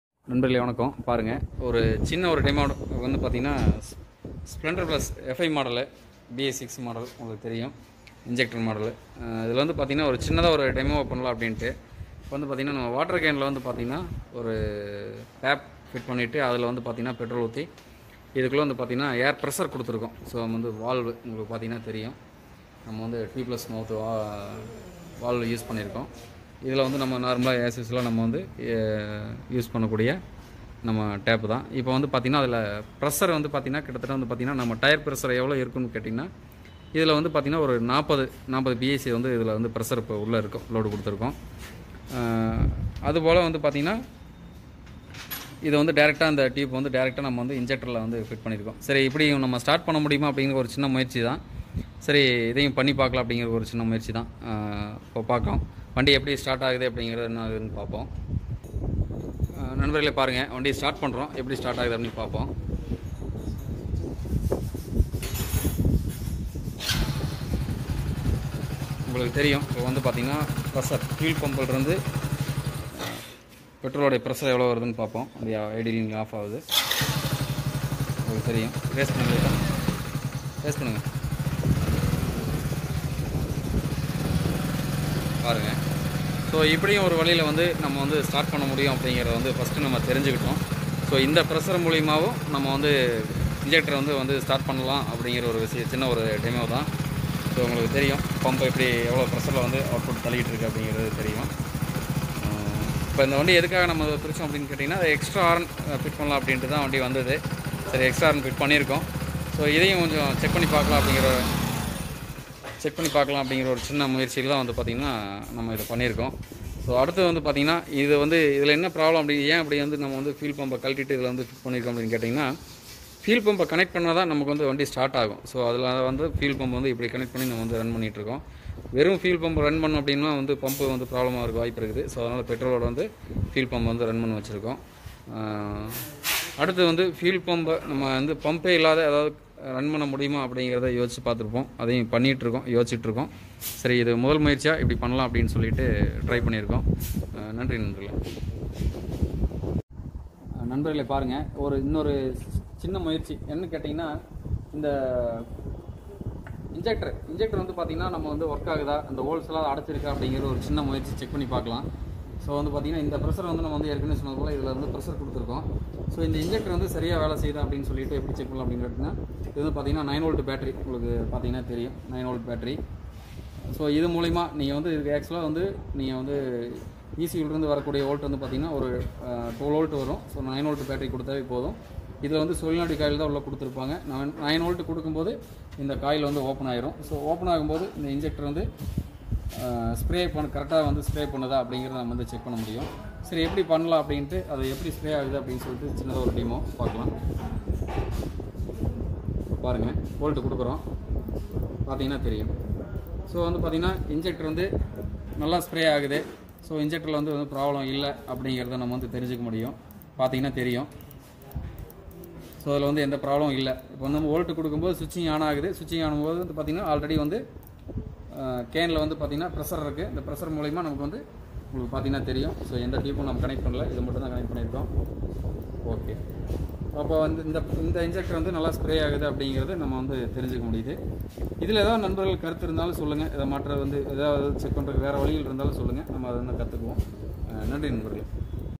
BS6 without fuel pump engine will start|ஒரு புதிய கண்டுபிடிப்பு
இ து ஒரு புதிய கண்டுபிடிப்பு.  முயற்சி செய்ய நினைத்தால் மிகுந்த பாதுகாப்பு தேவை.
தரமான தண்ணீர் பாட்டில் அவசியம்.

Its a new invention. prevention is very important if u try this.  water can quality is very important because of the pressure.